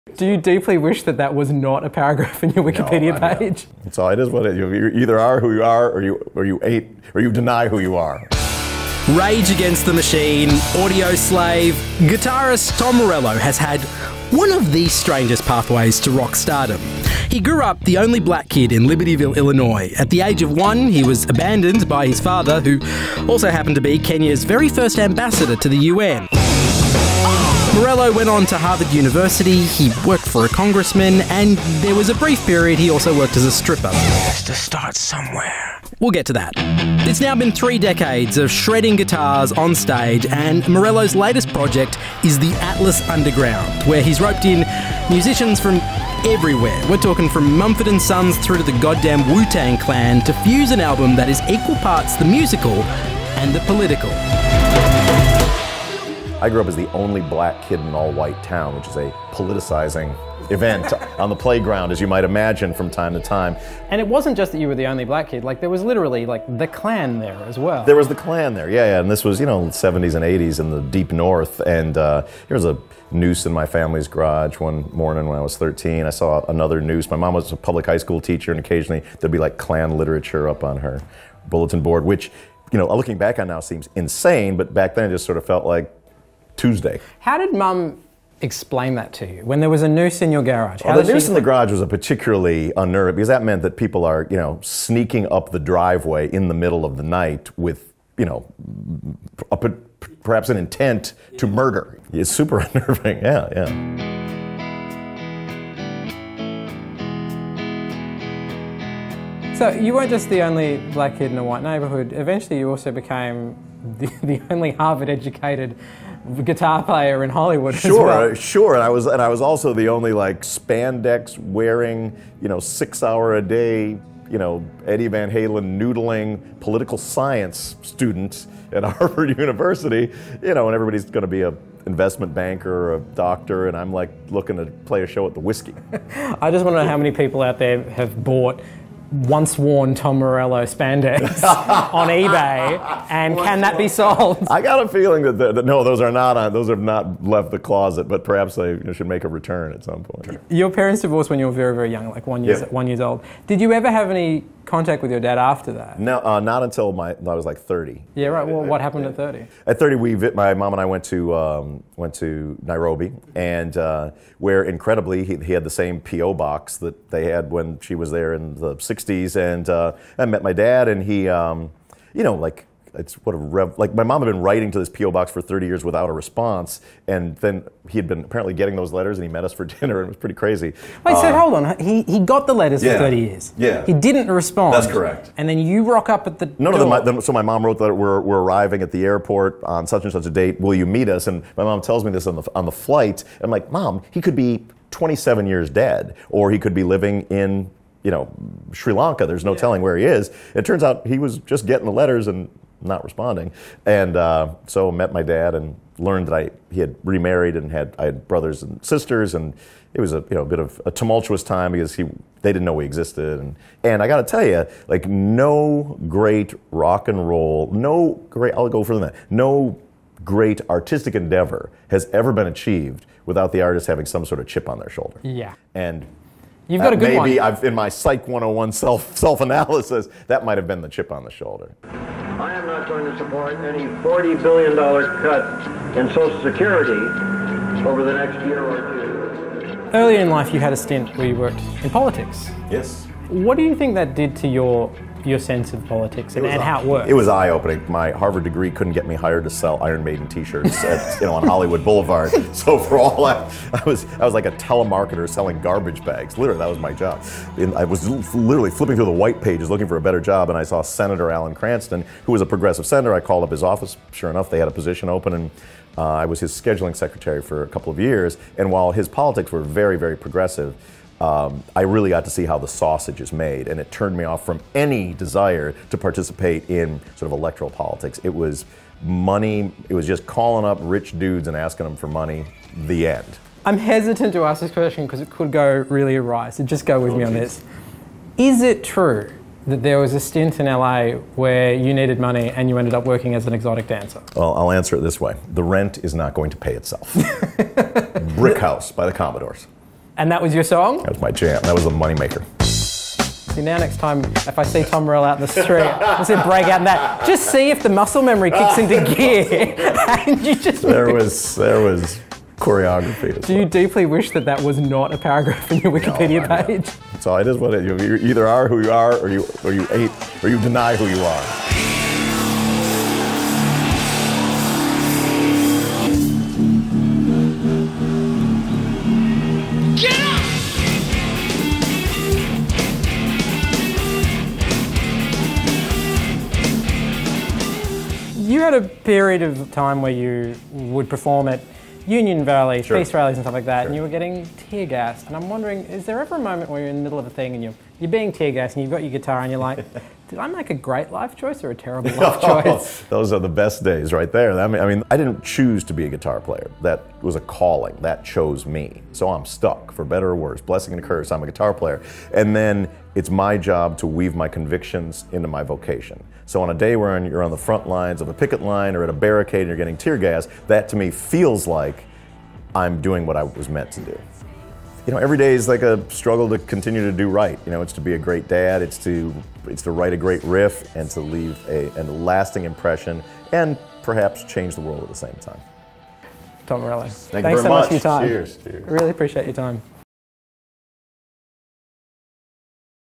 Tom Morello: On the front lines with Rage Against the Machine
He’s the guitarist of bands like Rage Against the Machine and Audioslave who’s no stranger to getting tear gassed. But Marc Fennell finds out about Tom Morello’s weirdest jobs along the way to stardom.